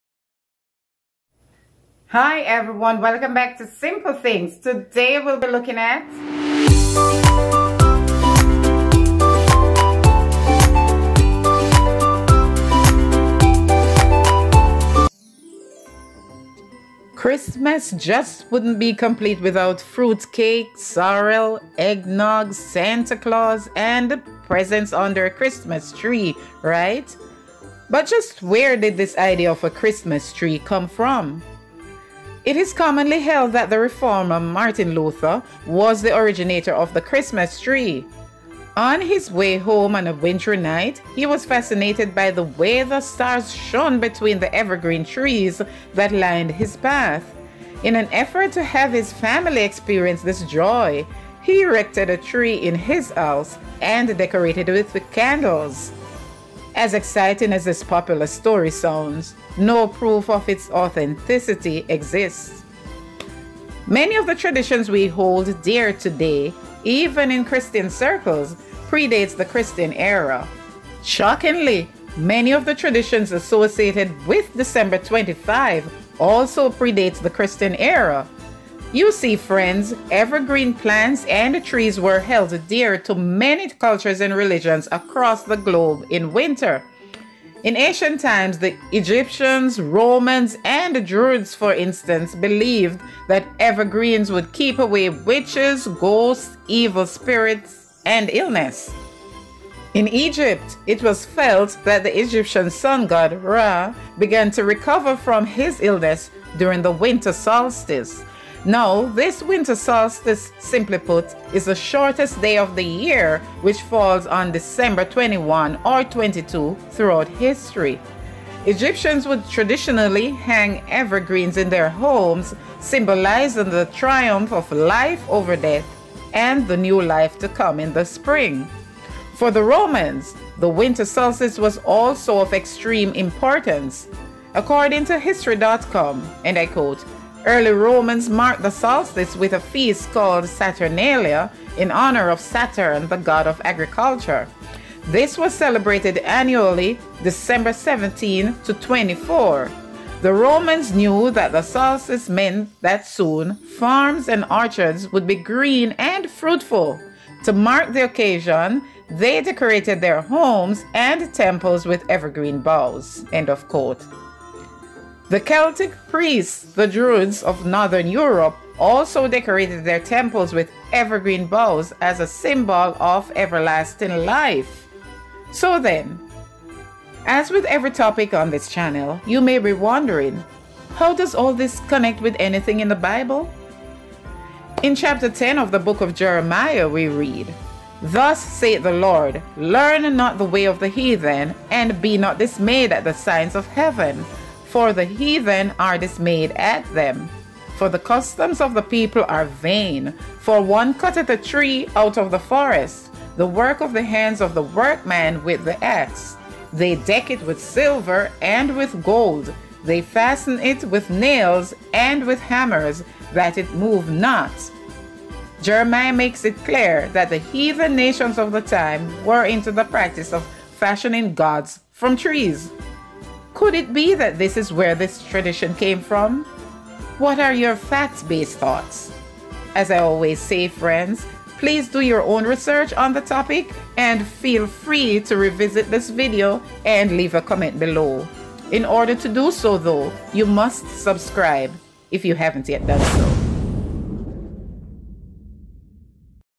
History of the Christmas Tree and the Bible
Exploring the origin of the Christmas tree tradition.